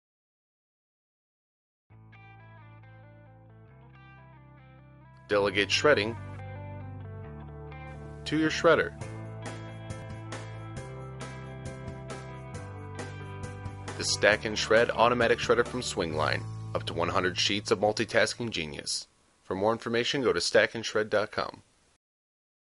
Swingline Stack-and-Shred © video 6
cut, automatic shredder shreds up to 100 sheets
at once so you can spend less time feeding the
shredder and more time getting things done.
Simply stack, shut and go.